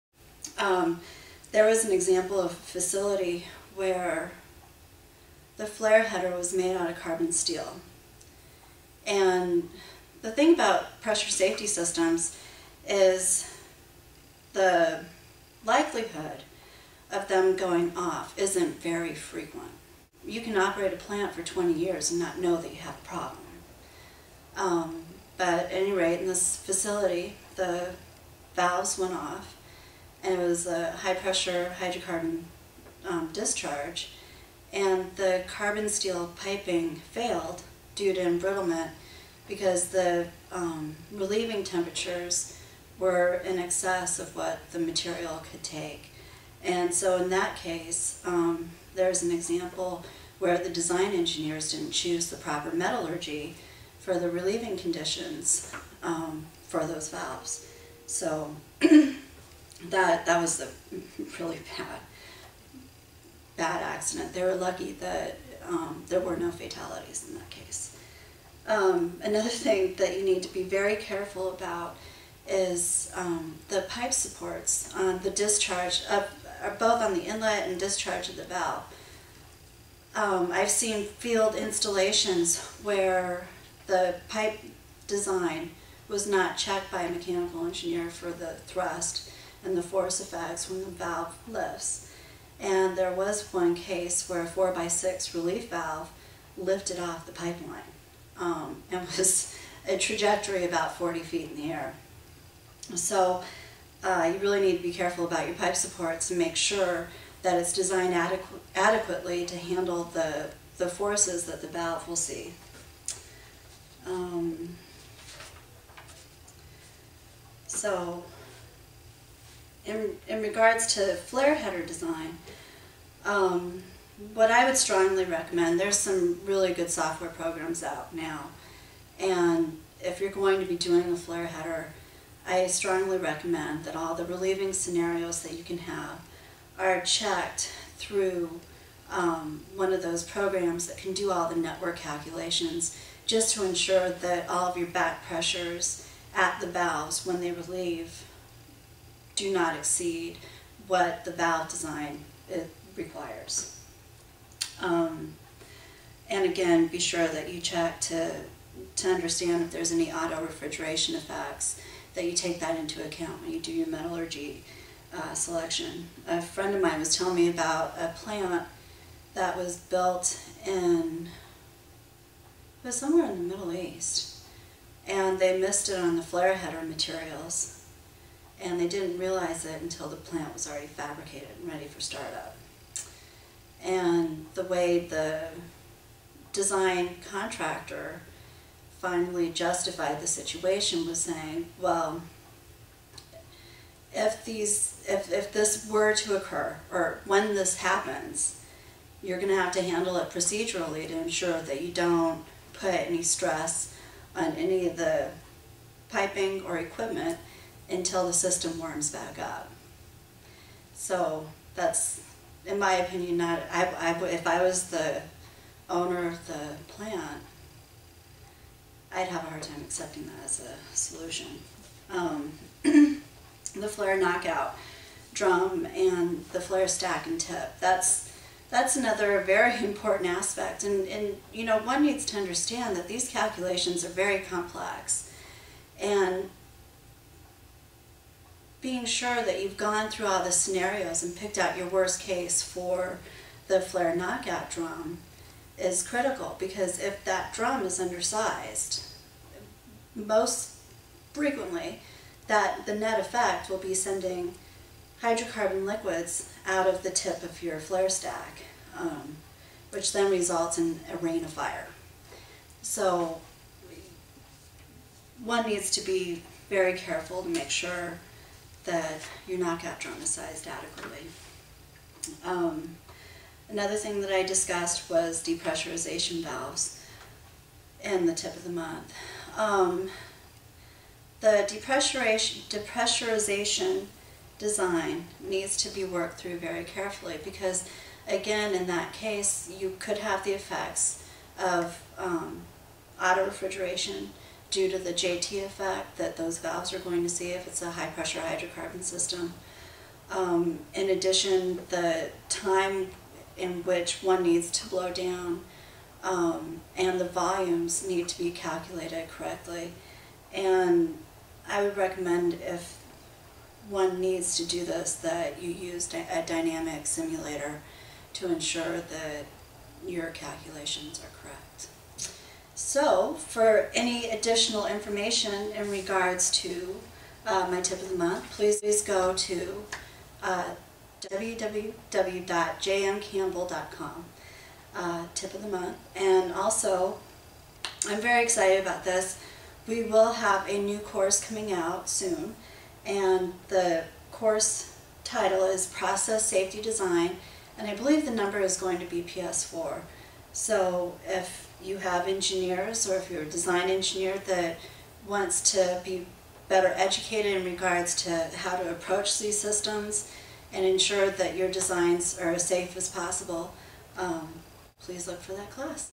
Pressure Relief System Design Pit-falls Part 2
Kindra Snow-McGregor discusses her popular Tip of the Month, Pressure Relief System Design Pit-falls. Part 2 of 2.